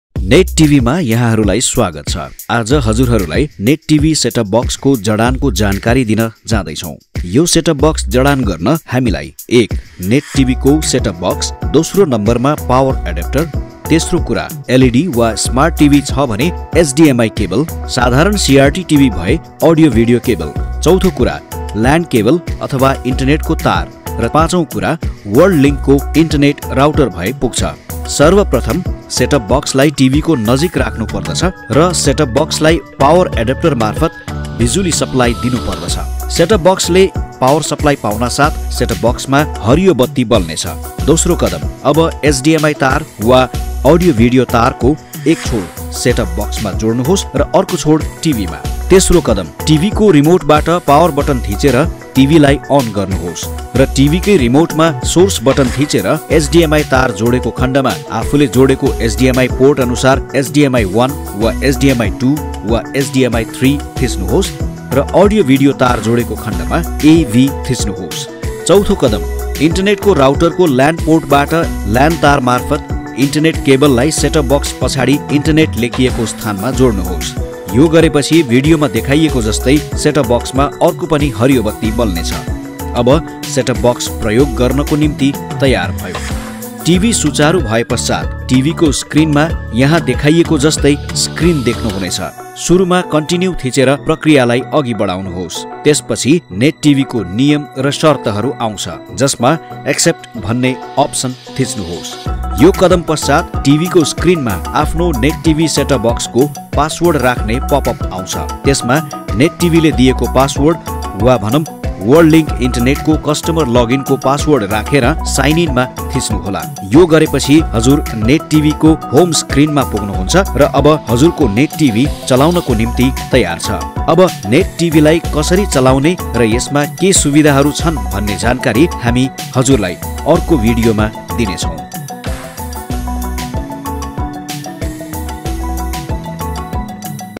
About NETTV Set Top Box Connection
This video will help you to set up your NETTV STB and identify and resolve minor issues.